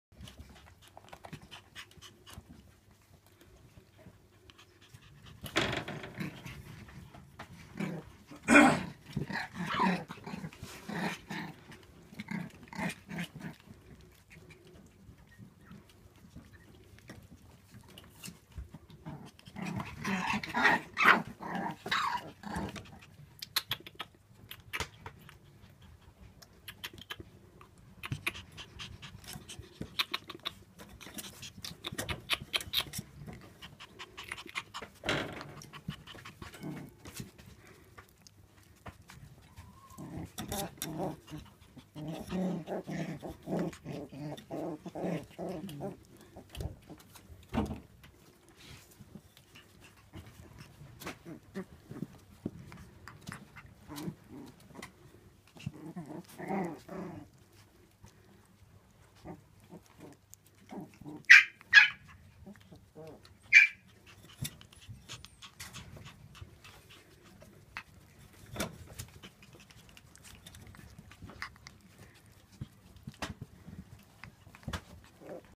柴犬総動員（笑） シベリアンハスキー＆柴犬専門₋WOLF CROWN₋
2016/05/08生　当犬舎の柴犬の子犬達です。
可愛すぎて困ります（笑）

まだまだ、柴犬の子犬のご家族様募集中。

今月出産予定
柴犬　茶々　6/26以降

来月出産予定
シベリアンハスキー　ロゼ　7/7以降

シベリアンハスキー&柴犬専門犬舎
-WOLF CROWN-